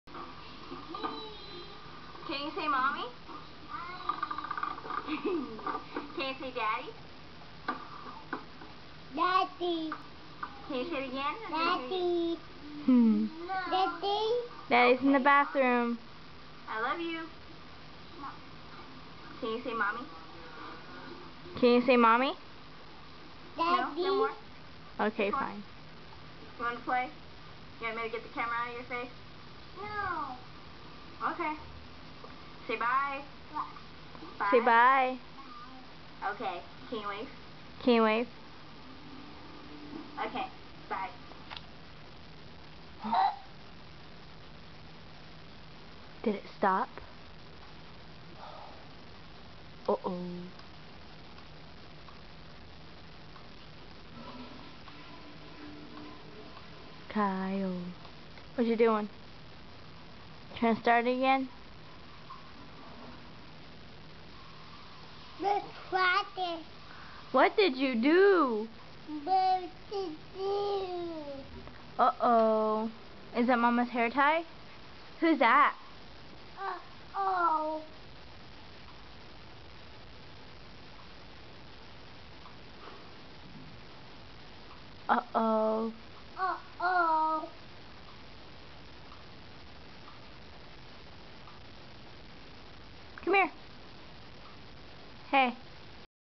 Kyle watching the video of himself saying Mommy
Kyle watching the video of himself saying mommy, and then saying daddy